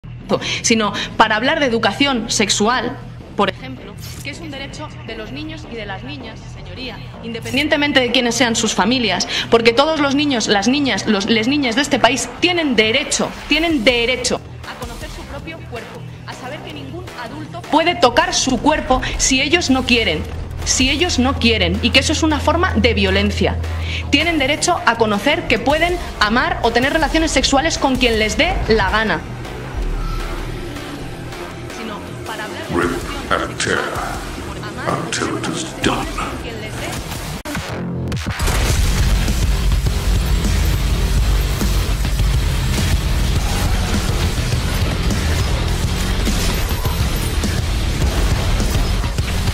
EU_ODEIO_O_ANTICRISTO.MP4
Video meme sobre a declaração da ministra do governo da igualdade da espanha no qual defendeu que menores possam ter relações sequiçuais de maneira consentida entre si e com adultos, id est, apoiando os somelieres de criança.

BTC: 1DYhseiUvfQBENJ43mGJFTXPanNa9RiPsz
ETH: 0xc7eD5BC35318EdB1aa4C58711366E19392eE2048
BCH: qzy6pps9xcc6yvdwtskztemtf76edr9vcsth8p8hq0
LBC: bRCUe9v1DdycQZLnc2ELxpFXj13iTVDt8H